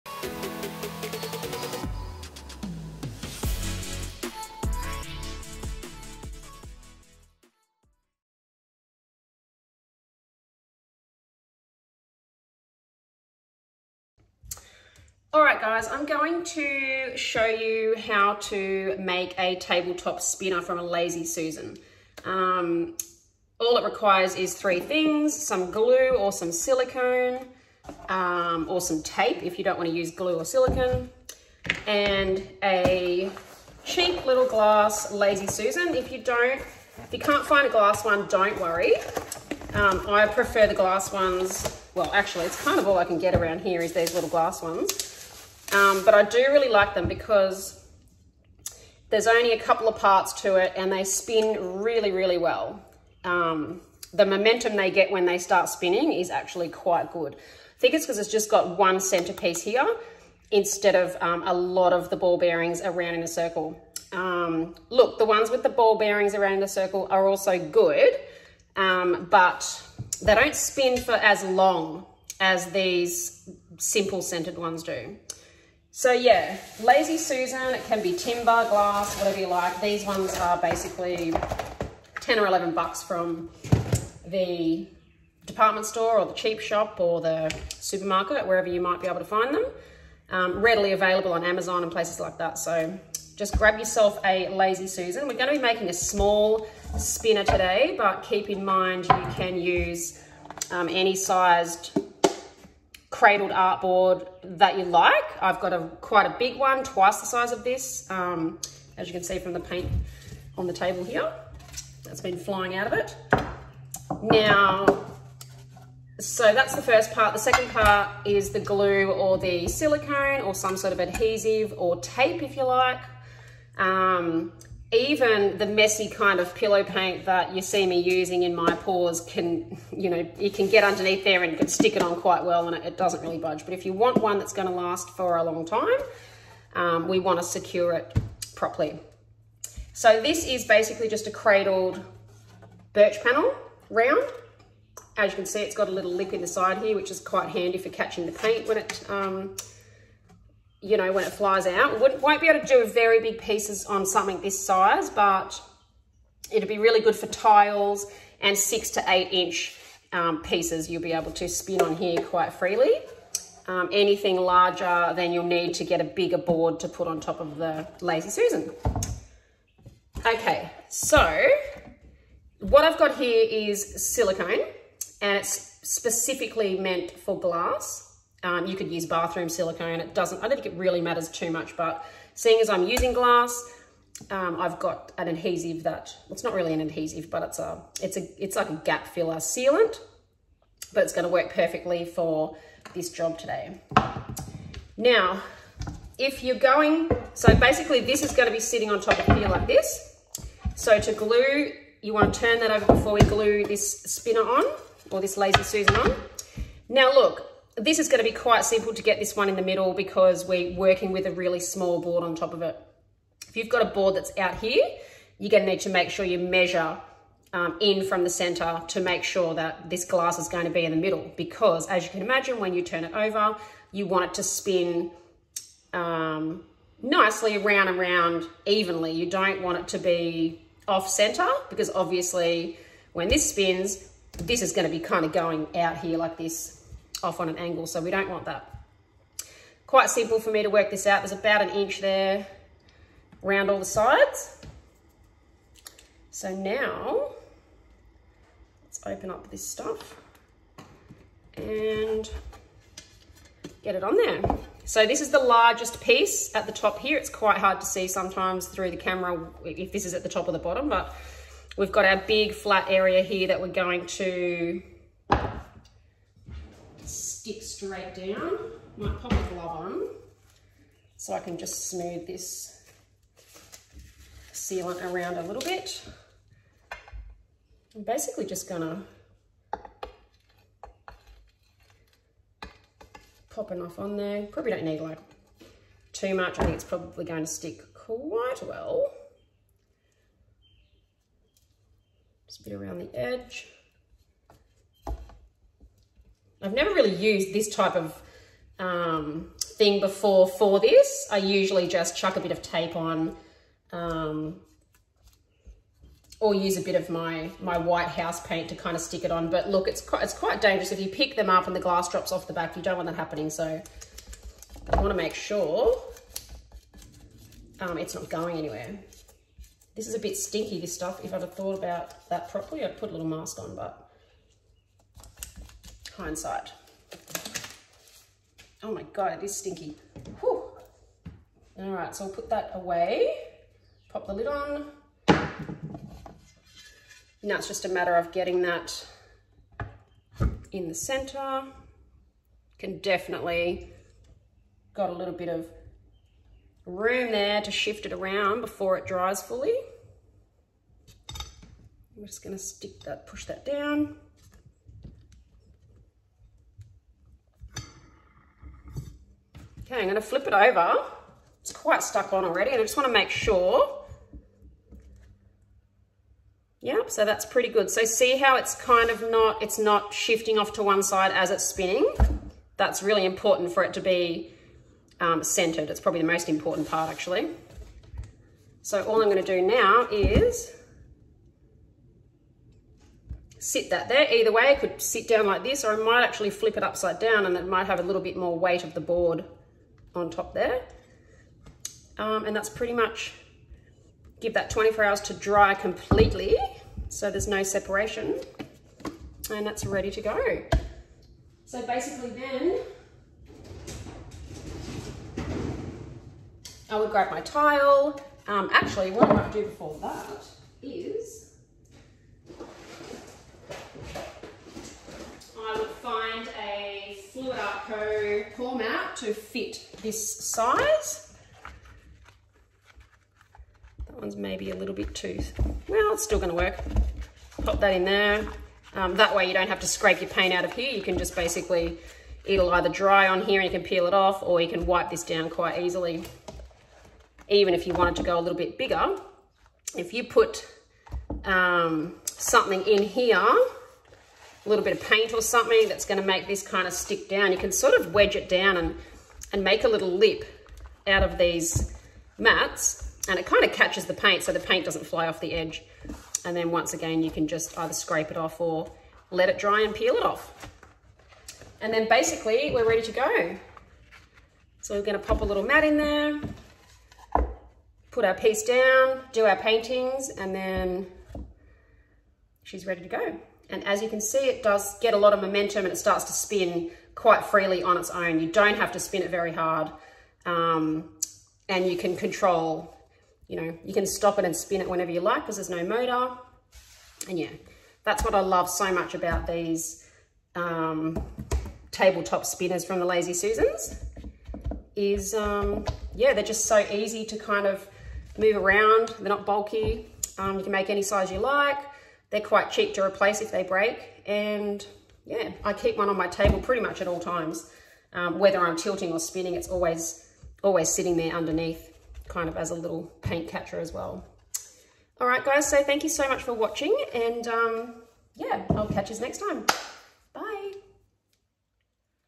229.Sheleeart - Fluid Artist. This is the video you've all requested. Tabletop spinner tutorial
I've hesitated to do this video, as I wasn't sure if it was too basic to be of any value to everyone. But instead of trying to explain it in words,  I have filmed a super basic video to show you how I make my spinners.   This method is cheap and easy and you only need 3 items to make one.  Hope you enjoy!